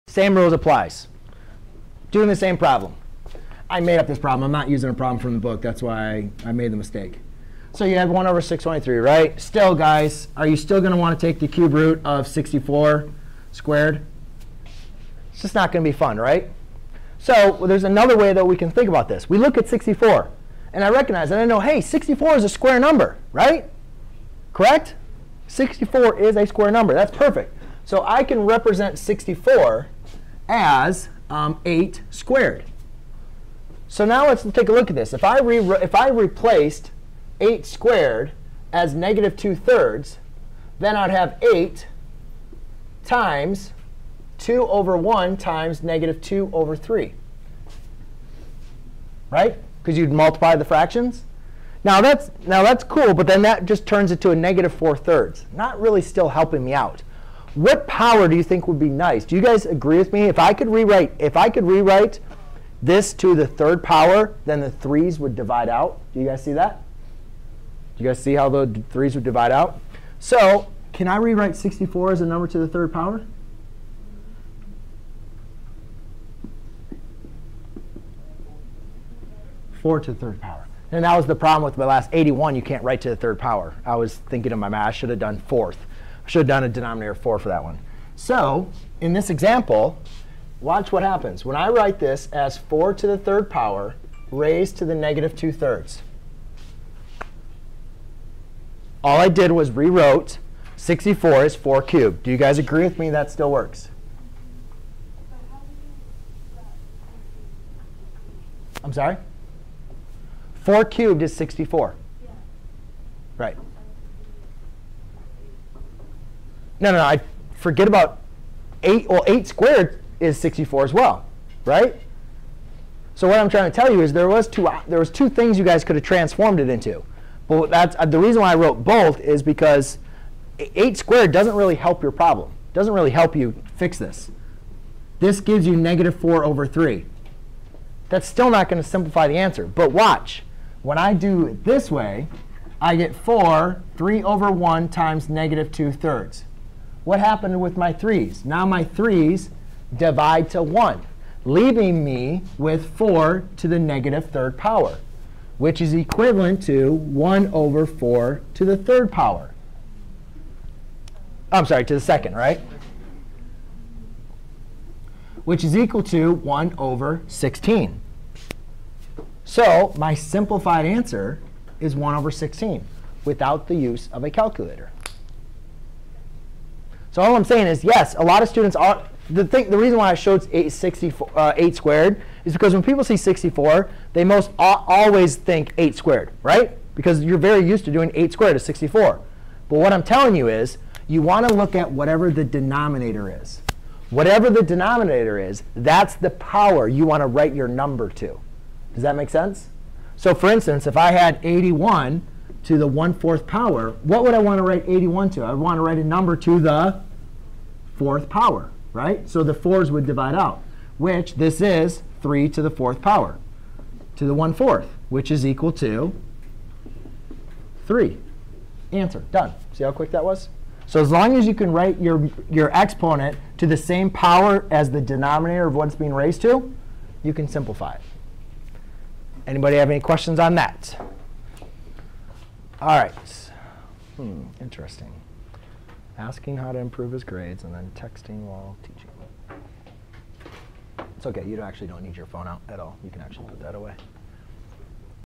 Simplifying an exponent with a negative rational power
Learn how to evaluate numbers raised to rational powers. When given a number raised to a rational power, we take the nth root of the number where n is the number in the denominator of the rational power, then we raise the result to a power equivalent to the number in the numerator of the rational power. i.e. x^(m/n) = nth root of x raised to the power of m.